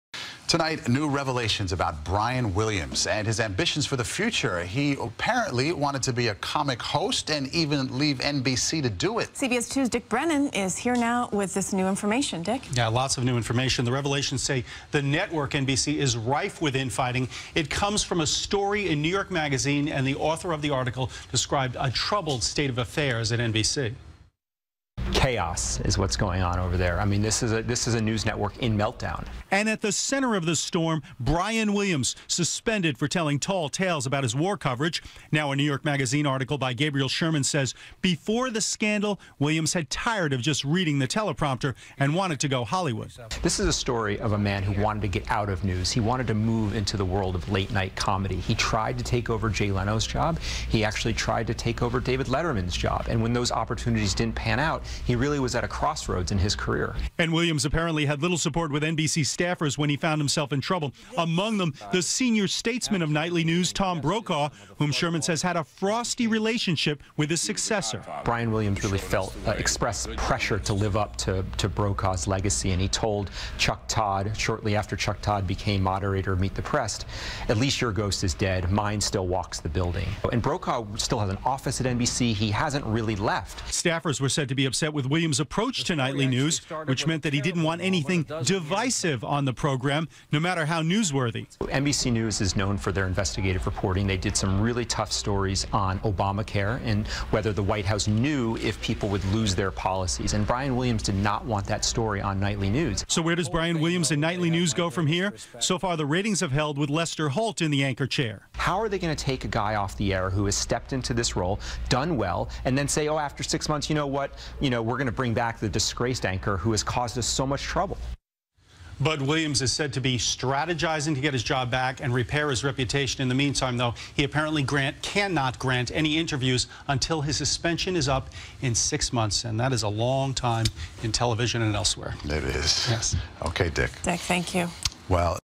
Brian Williams Apparently Had Comedic Aspirations
The anchorman wanted to be a comic host, and was ready to leave NBC to do it. CBS2's Dick Brennan has more.